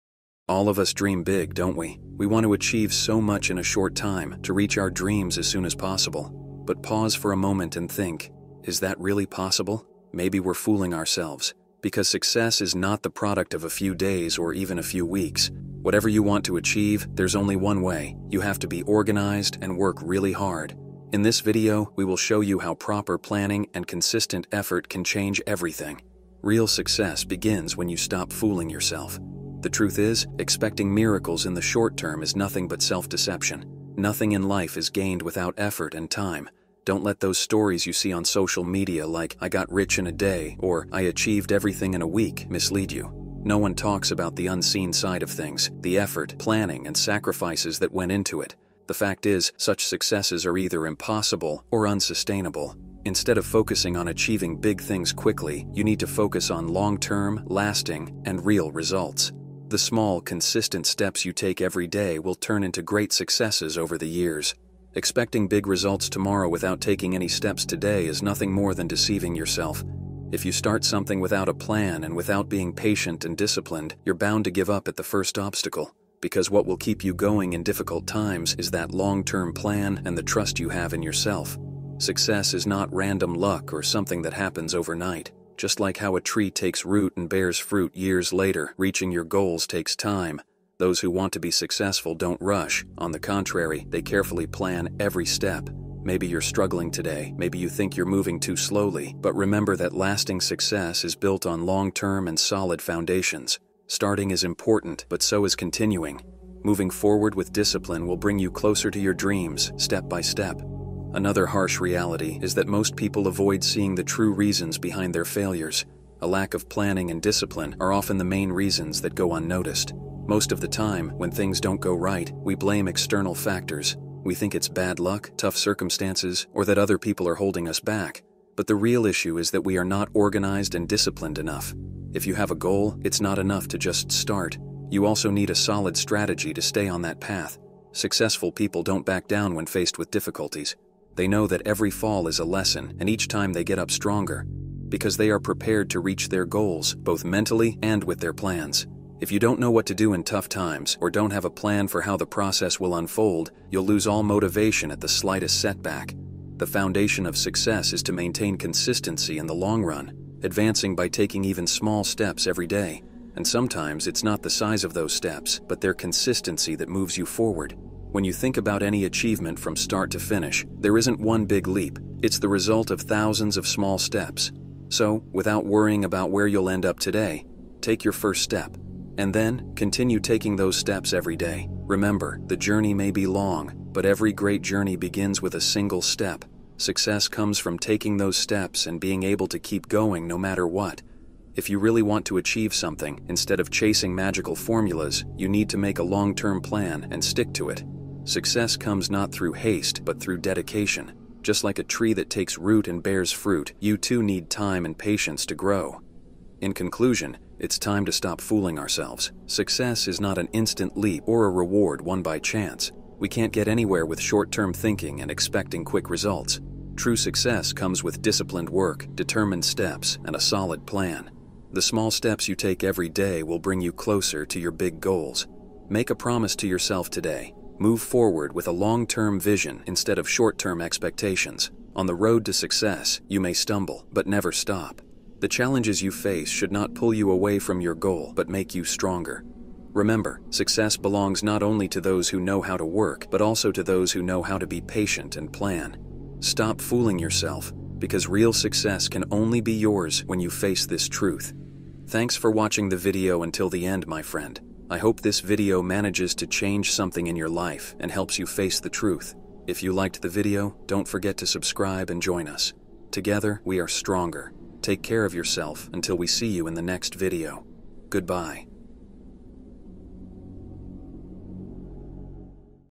Stop Fooling Yourself and Sabotaging Your Own Success.
Stop fooling yourself and sabotaging your own success. All of us dream big, don’t we? We want to achieve so much in a short time, to reach our dreams as soon as possible. But pause for a moment and think... Is that really possible? Maybe we're fooling ourselves. Because success is not the product of a few days or even a few weeks. Whatever you want to achieve, there’s only one way: You have to be organized and work really hard. In this video, we will show you how proper planning and consistent effort can change everything. Real success begins when you stop fooling yourself.